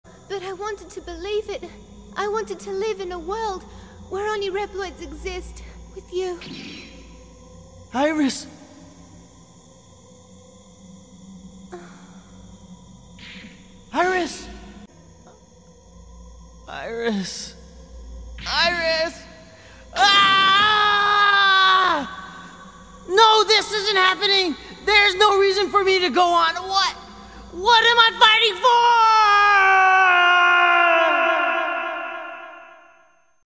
[Read Desc.] I hate the game awards
Note that my opinions back then aren't accurate right now
Eldin ring clearly deserved the trophy over a bland and linear playable movie
but I'm still confused as to why a literal 2020 game won over a game that's released in 2022.